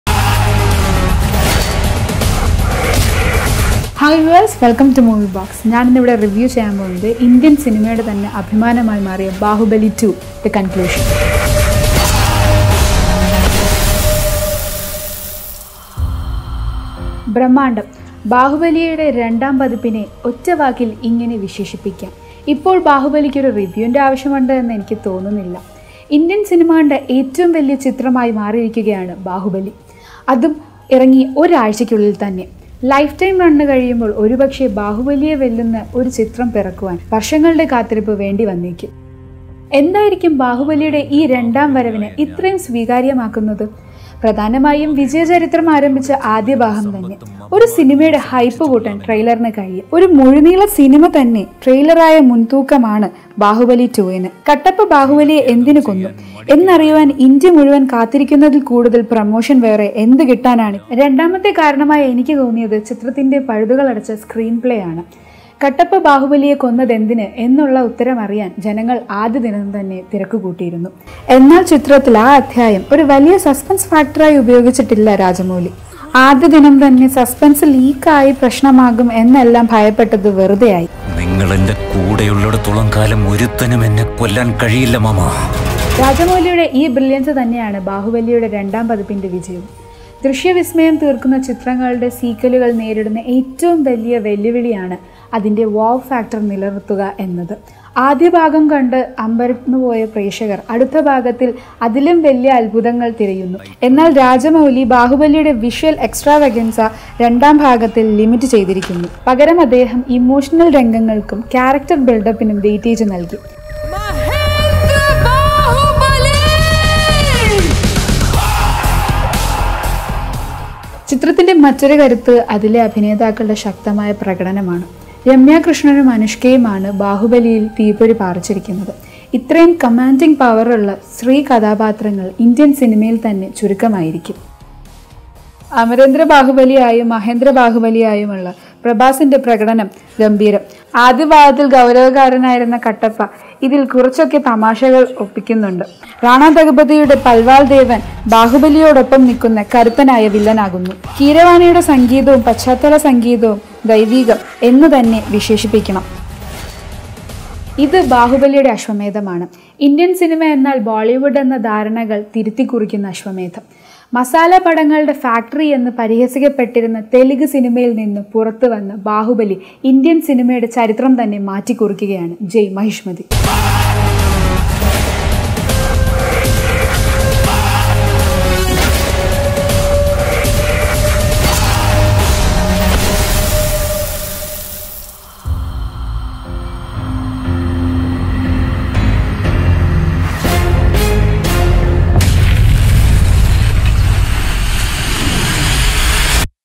Baahubali 2: The Conclusion - What makes Bahubali the top grossing film of the Country?
Baahubali 2: The Conclusion Movie Review || Movie Box || Prabhas || Anushka Shetty || Rajamouli